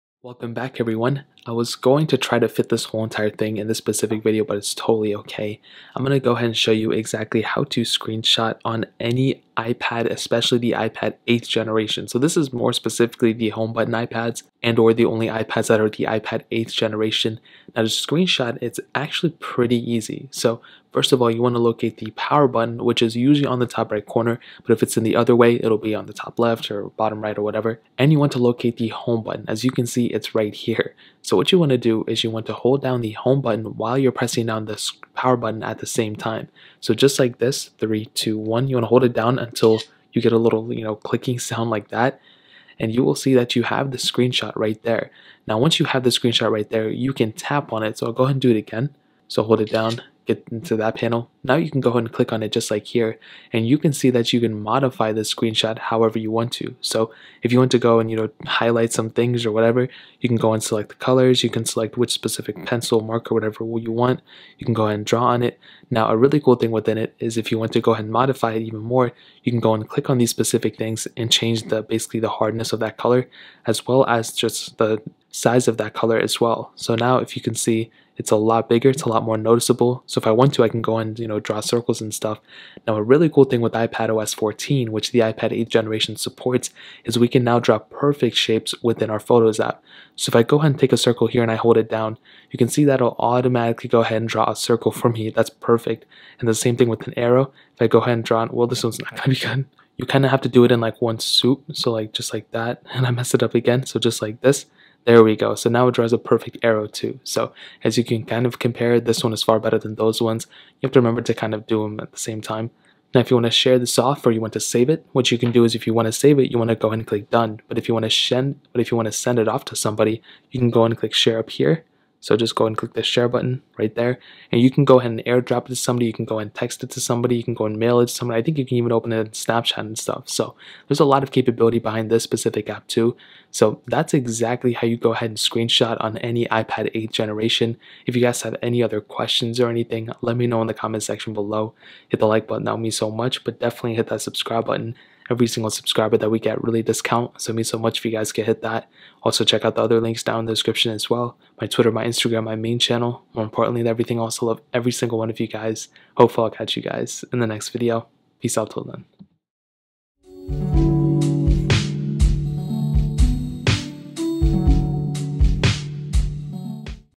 How To Screenshot On iPad 8th Generation!
Here is exactly how to screenshot on the iPad 8th Generation!